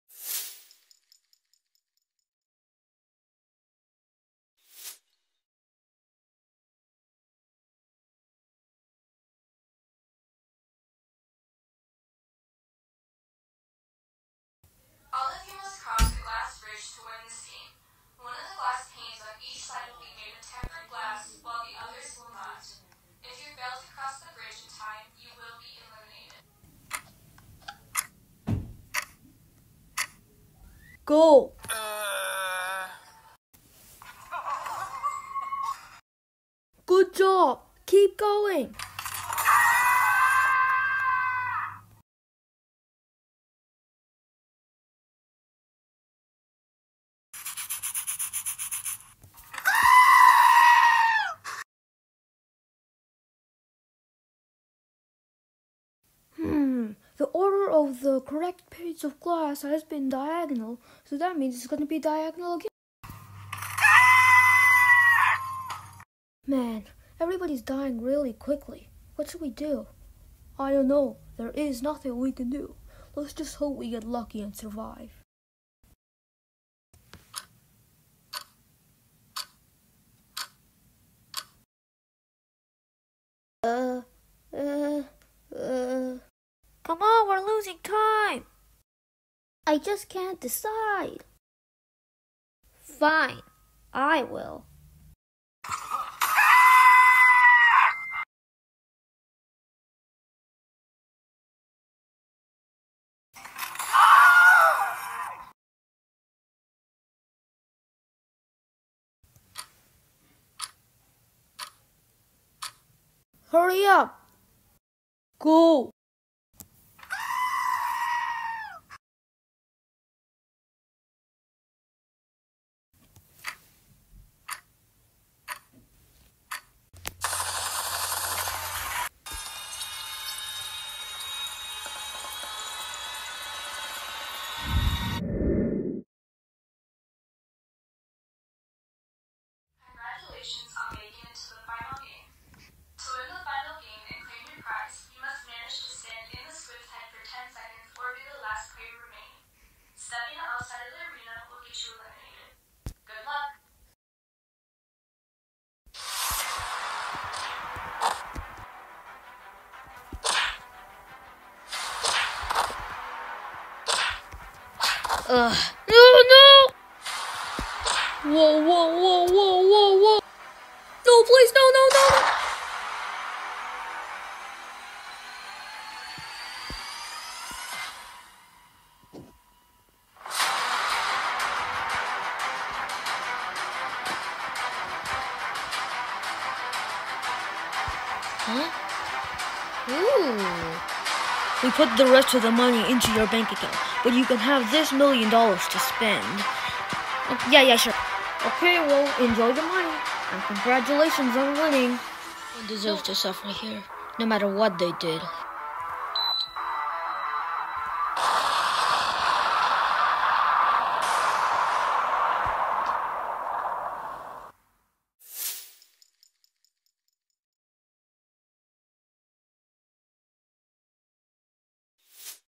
Glass bridge stop motion I made 2 years ago
I rembember I used t be so proud of this, but no it looks like literal garbage.